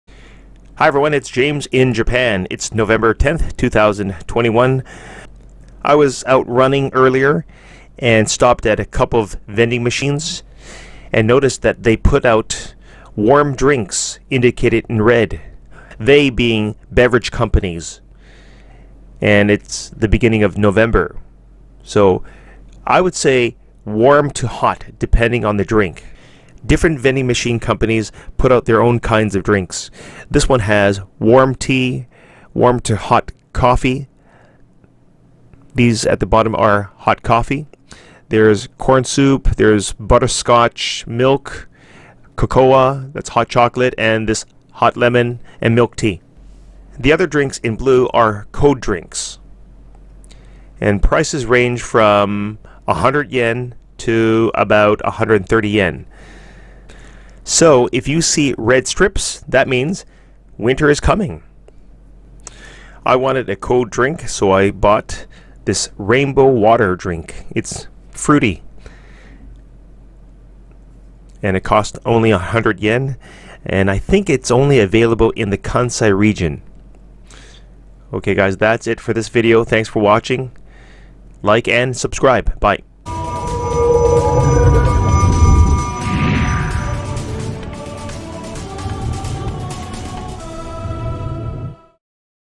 OMG! IF YOU SEE RED STRIPS AT JAPANESE VENDING MACHINE THEN YOU KNOW WINTER IS COMING! Nov 10,2021
Nov 10,2021

I was out running and stopped at a couple of vending machines and saw warm-hot drinks which means winter is coming.  Vending machines are ubiquitous in Japan and they provide a convenient and refreshing experience.